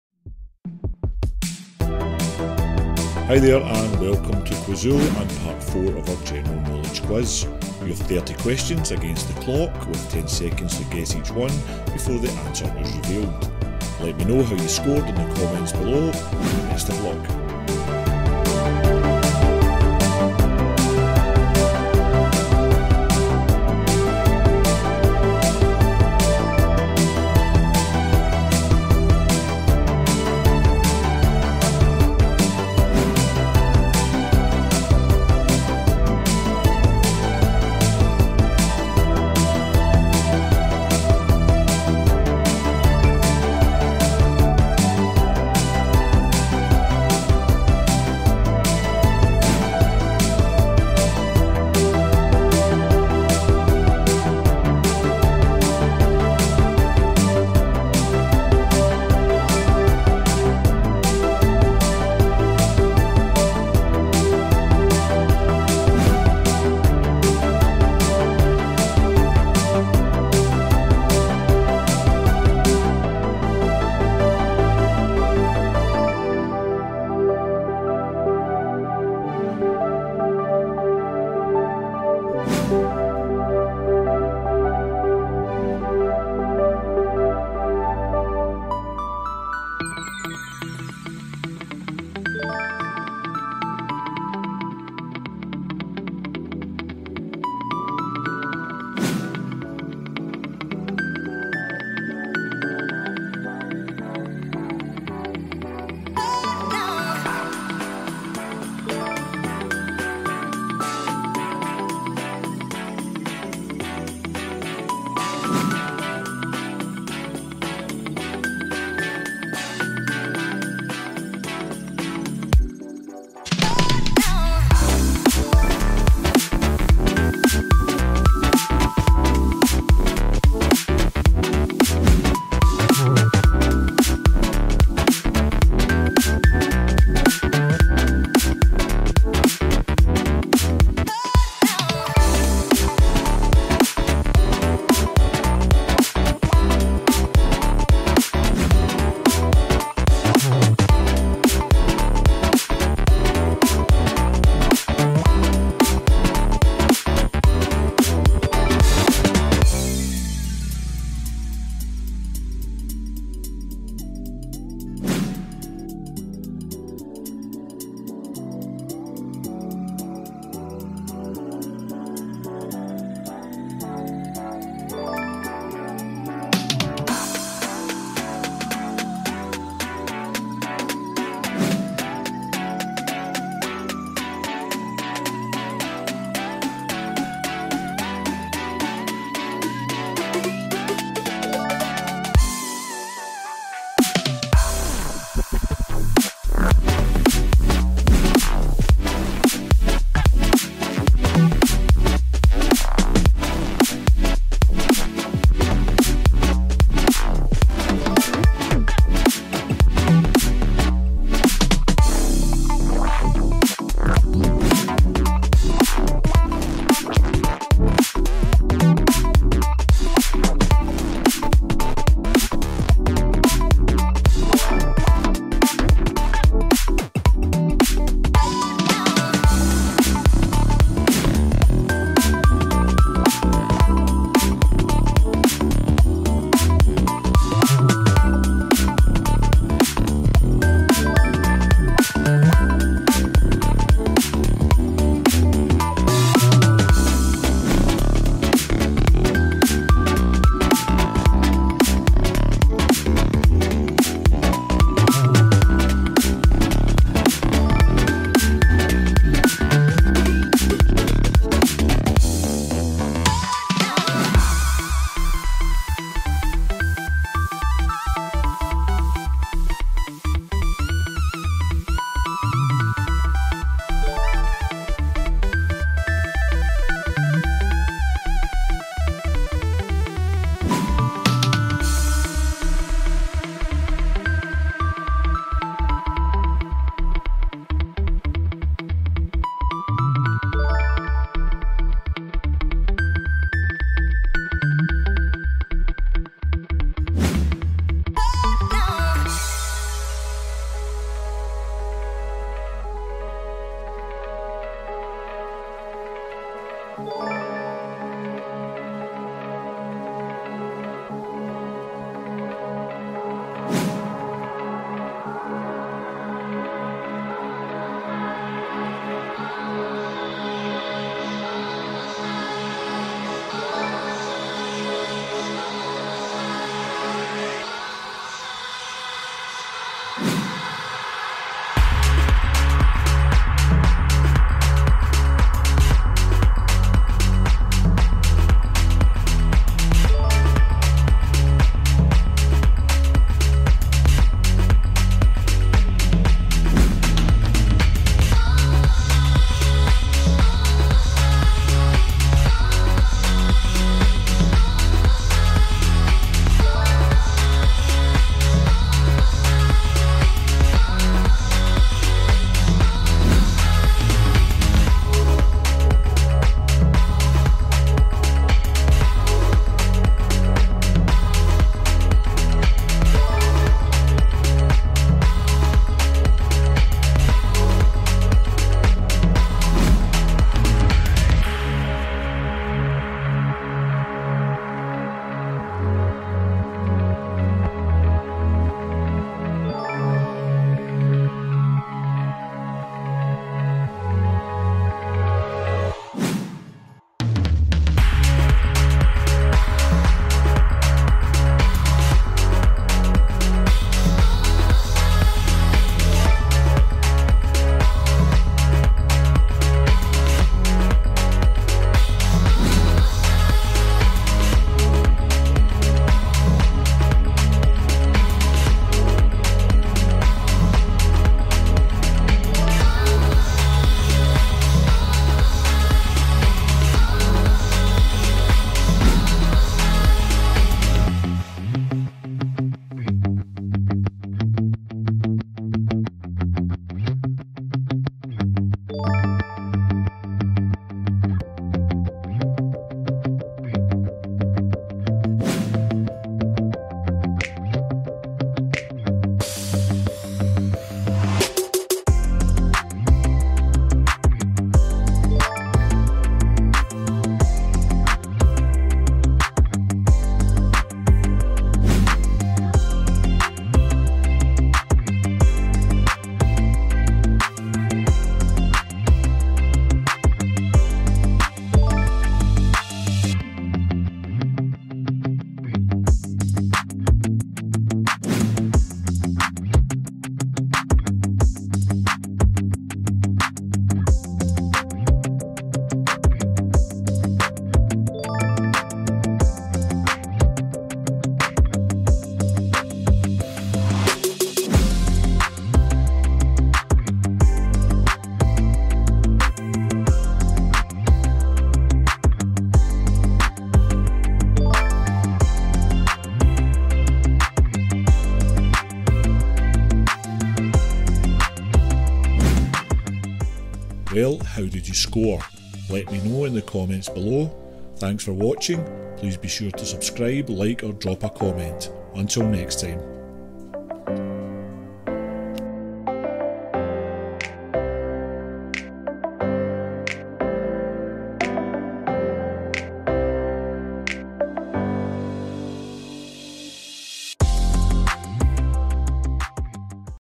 General Knowledge Quiz | 30 Questions With Answers #4
Today on QUIZOOL we give you part 4 of ''30 General Knowledge Questions with Answers. Do you know at which venue is the British Grand Prix held? or what name is given to hills and ridges composed of drifting sand?  If so, you're well on the way to full marks!